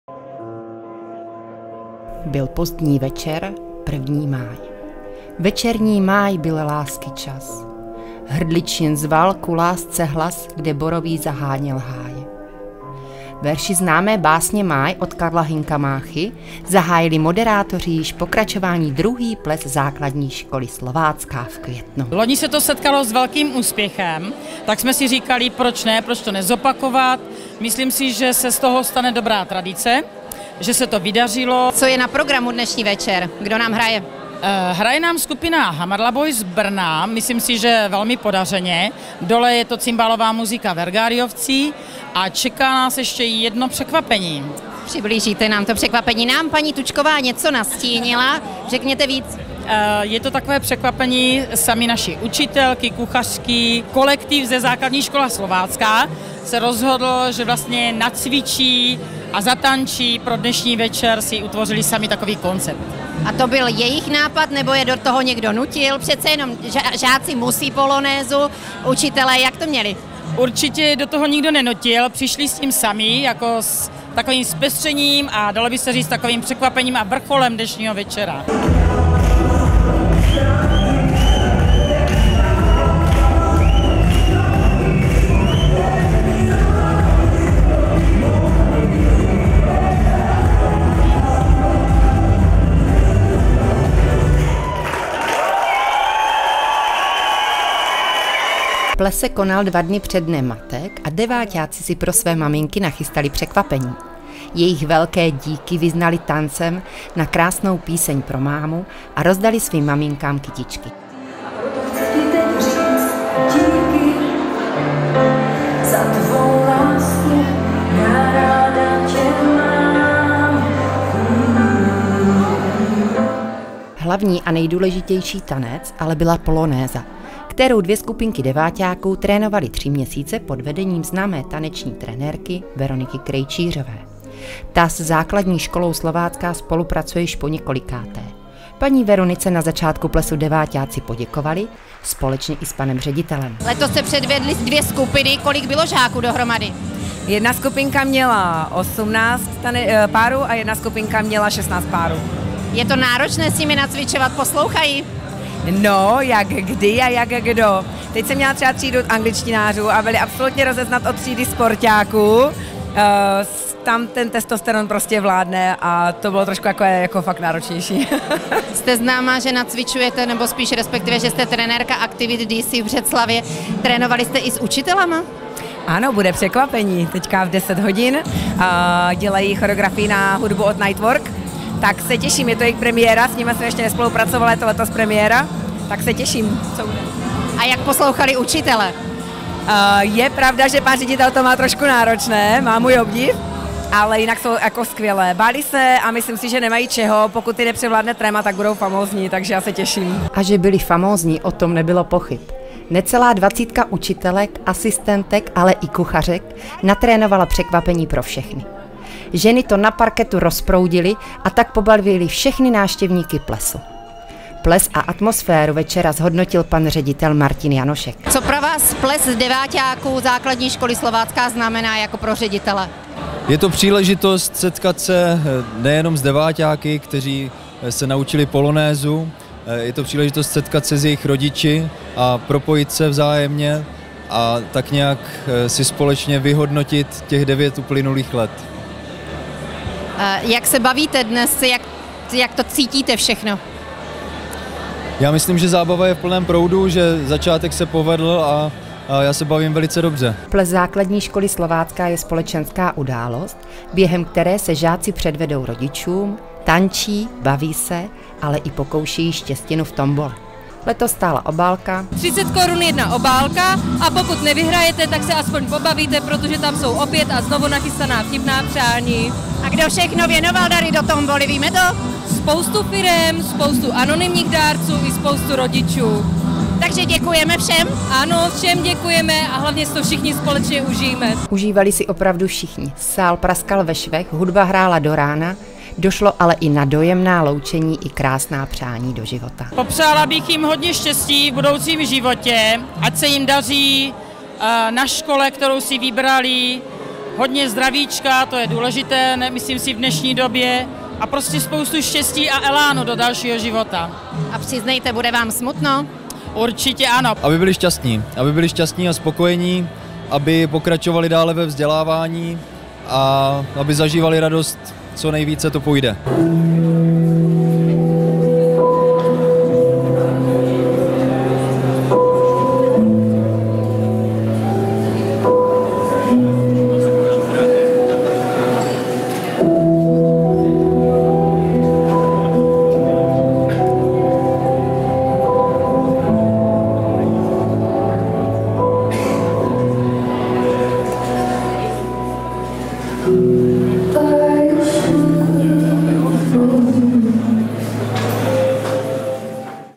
Byl pozdní večer 12. máj
Byl pozdní večer – první máj – večerní máj – byl lásky čas. Hrdliččin zval ku lásce hlas, kde borový zaváněl háj. Verši známé básně Máj od Karla Hynka Máchy zahájili moderátoři již v pokračování druhý ples Základní školy Slovácká v květnu.